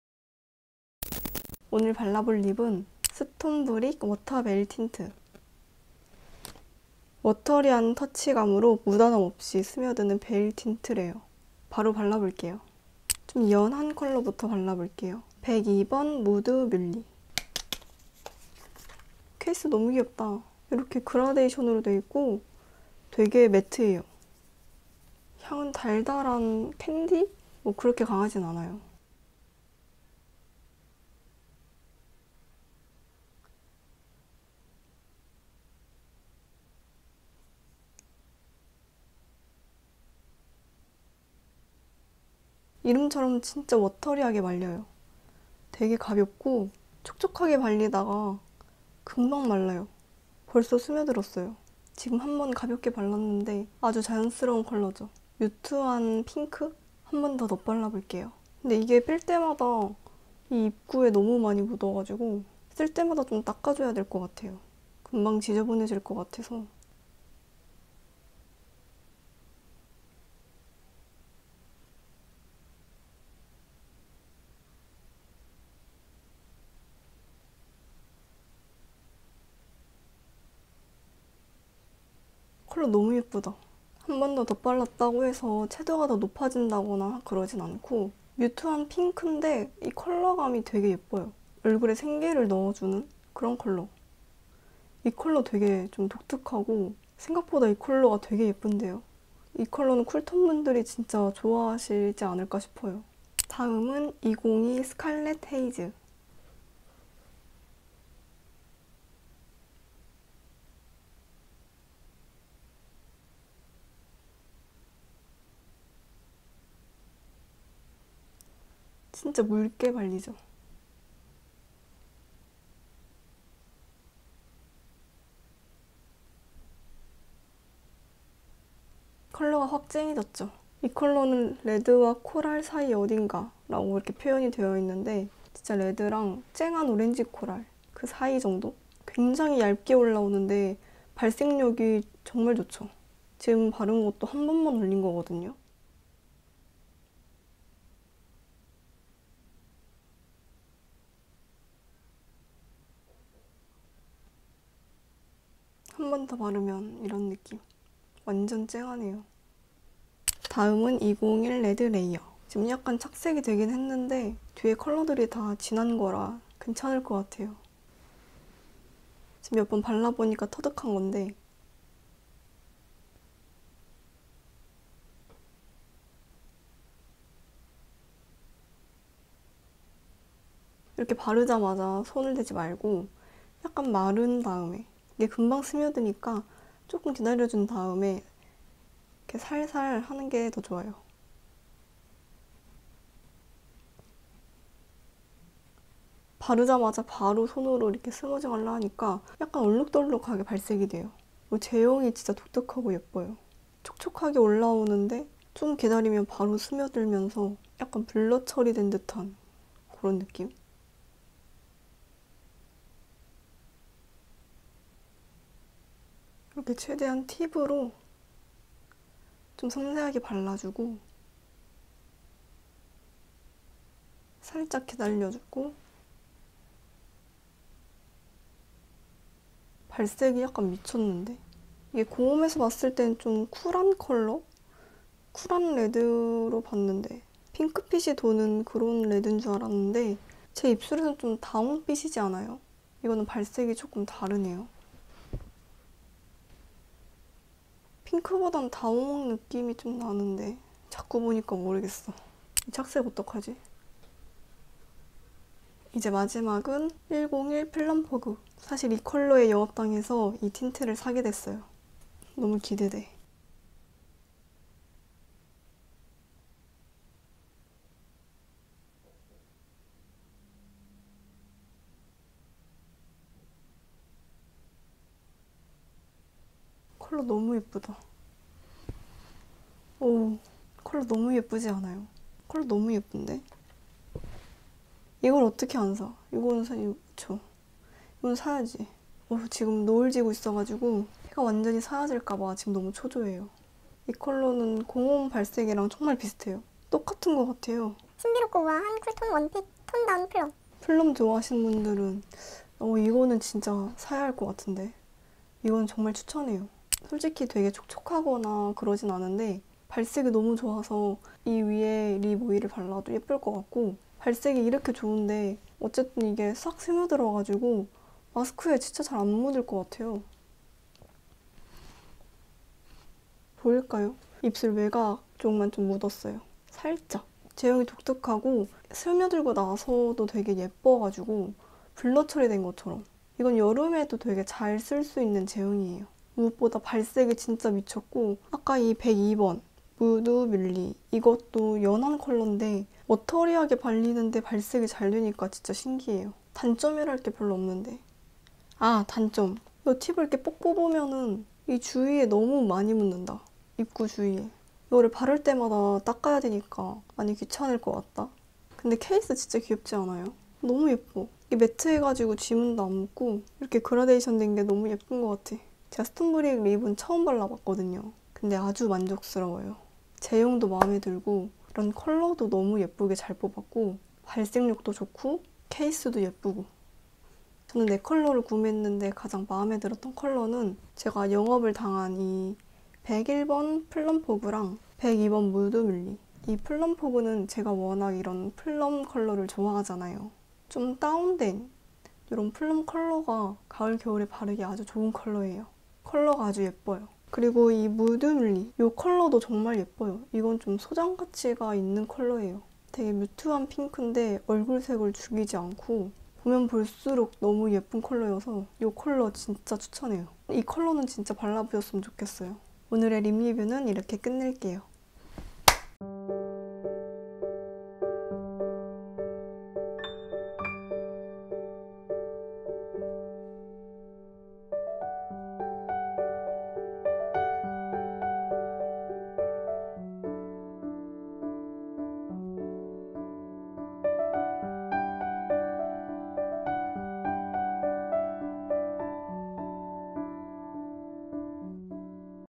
스톤브릭 마무리감이 정말 예쁜!! (플럼 존예💜) 워터베일틴트 발색/ stonebrick / water veil tint / 마스크에 안 묻어 나는 틴트 추천
(영상에서 꼭 수정할게 있어서 재업로드했어요,, 내 소중한 댓글 ㅜㅜ)

-영상에 나온 제품-
스톤브릭
워터 베일 틴트
101플럼포그/102무드뮬리/201레드레이어/202스칼렛헤이즈